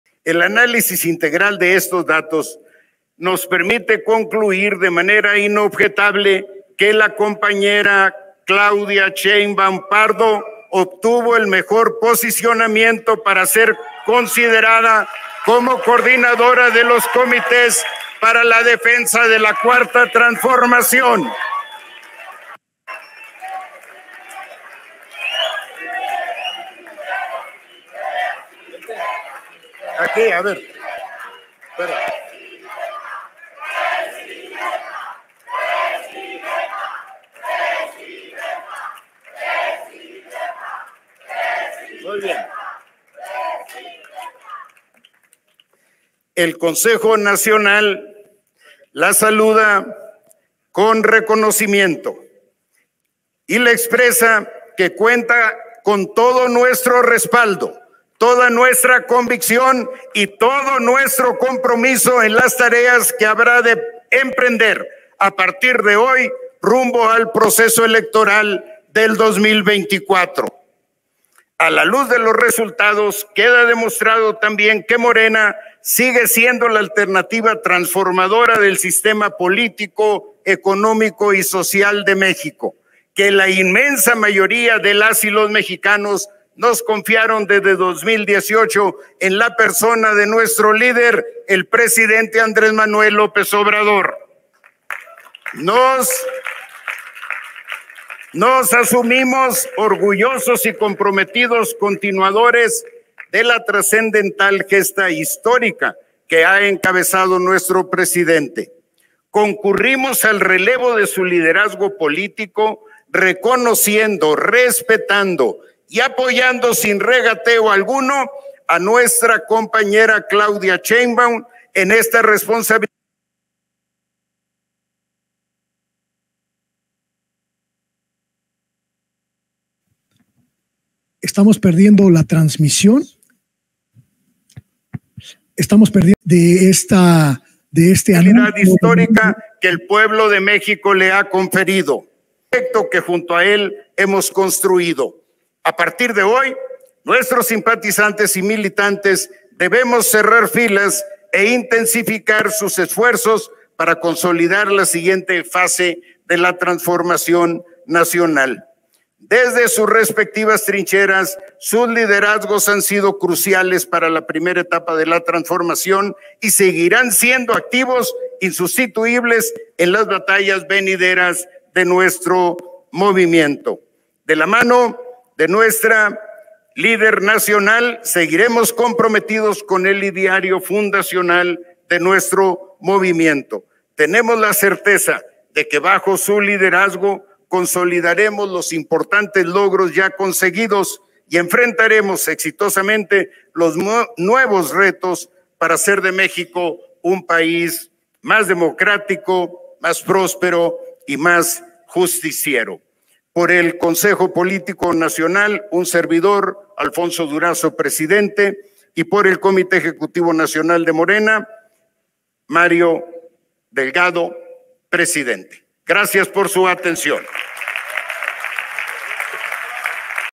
¡CLAUDIA SHEINBAUM ES NOMBRADA COORDINADORA DE COMITÉS PARA LA DEFENSA DE LA 4T!
Luego de dar a conocer los resultados de las encuestas, Alfonso Durazo nombra a Claudia Sheinbaum como la coordinadora de los comités para la defensa de la Cuarta Transformación